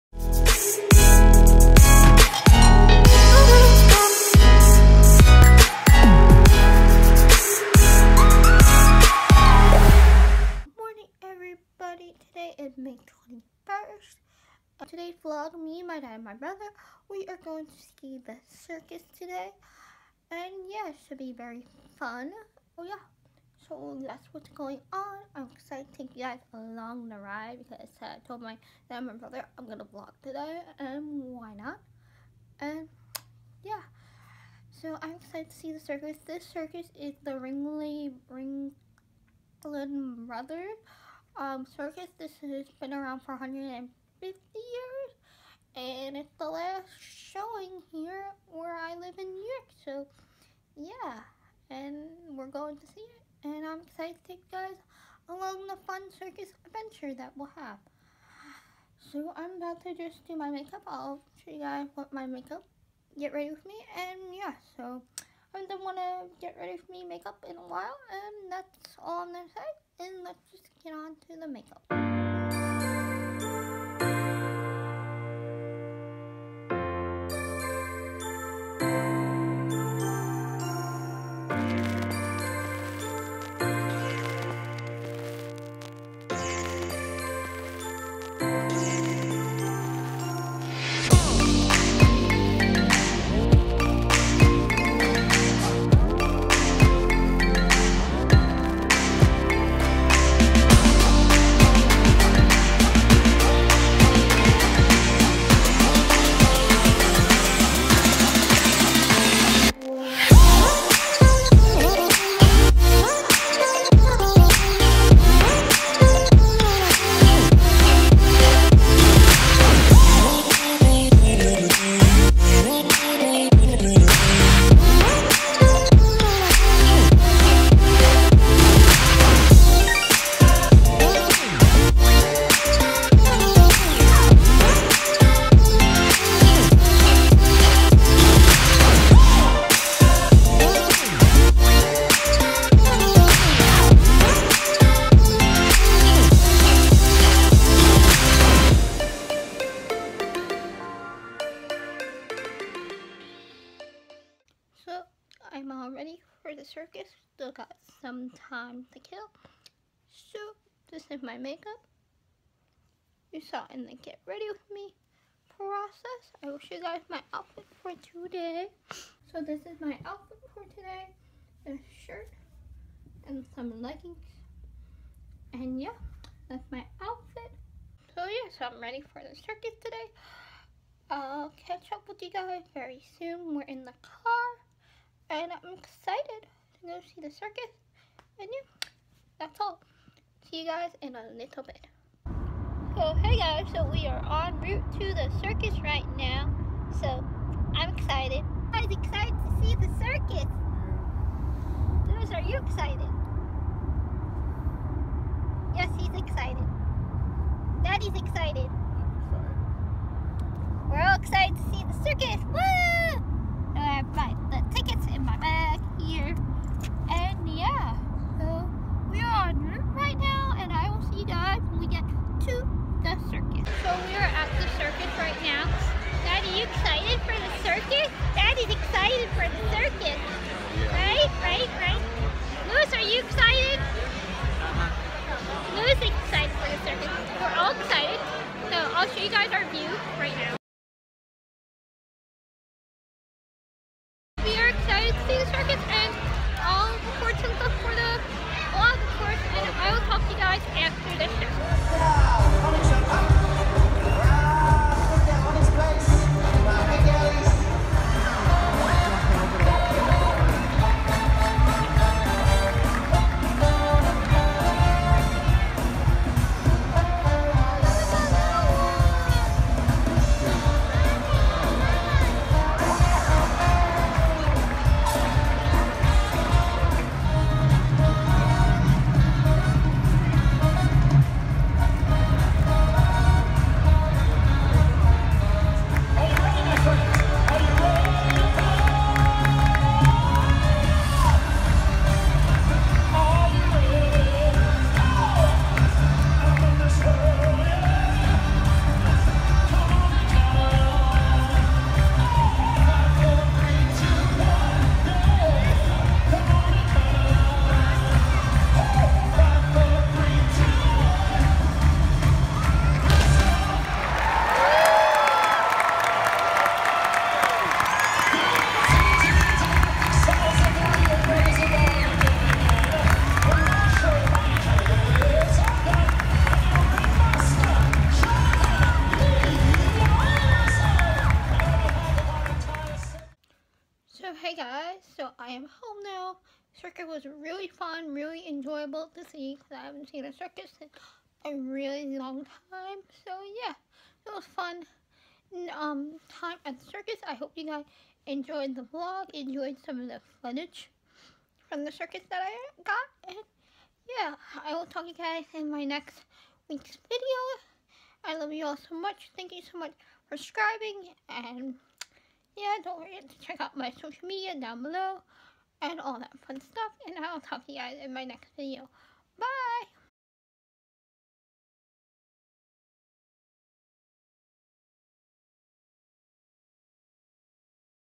Seeing The Circus //Vlog
Hey guys!! This week video me, my dad and my brother we all went to see the circus here in new york! I decided to vlog and take you guys along the adventure! Enjoy
Also more vlogs will be coming your way soon!

Editing hacks/green screen transitons used from

Edit: Imovie & Wondershare filorma
Film: Cannon t5i 18-55mm lens
xoxo Esther